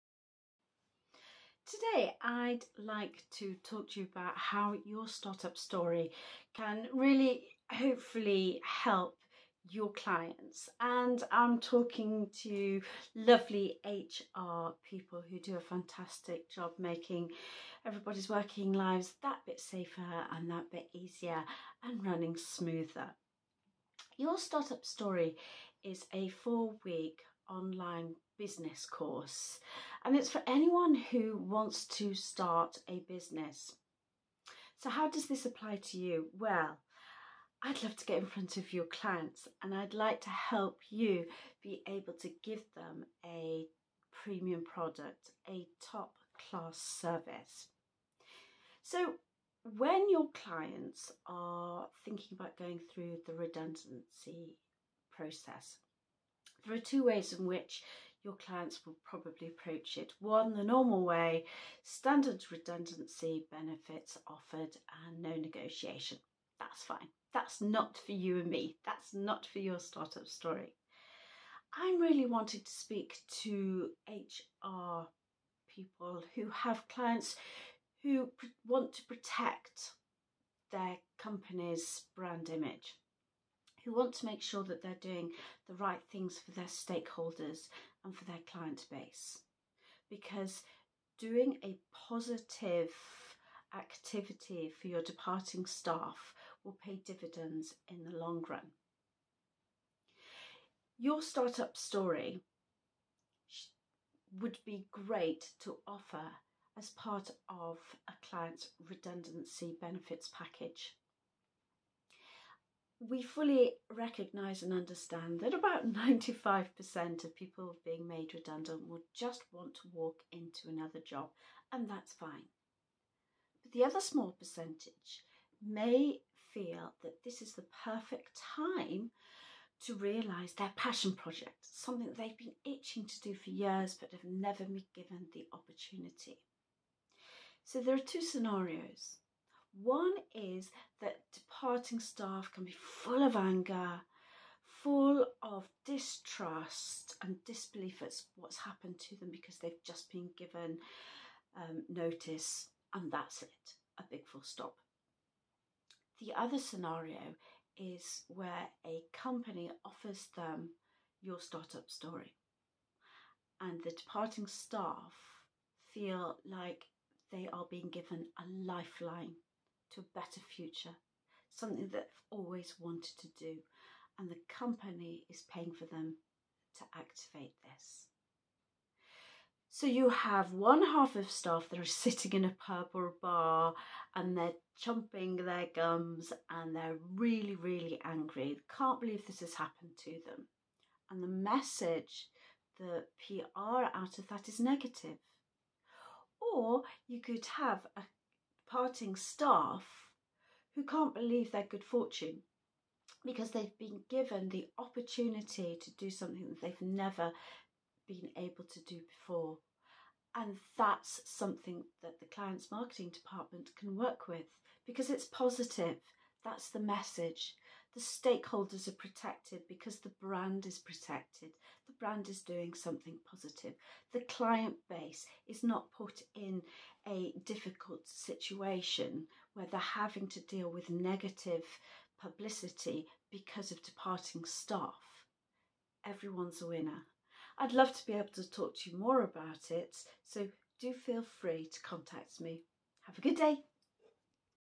Gold plated, bolt on Redundancy Benefits service for HR Consultants & Recruitment Agencies
Your Start Up Story offers a gold-plated, bolt on Redundancy Benefits service  are able to offer their individual, premium client base.

Understanding that most departing staff will want to step right into another salaried position, YSUS offers a uniquely designed course aimed at the minority of people who would like to explore working for themselves.

YSUS is an online, 4 week course with all the Guides, Workbooks and How-to Videos taking clients through the start-up process. The difference, however, is that YSUS is supported by on-demand, expert help from a start-up industry professional.

With over 20 years experience, Sofia Middleton guides, advises and hand-holds individual YSUS clients through the course. This course naturally evolves and firmly becomes, bespoke to the business owner and their new business.

for more info on incentive fees etc.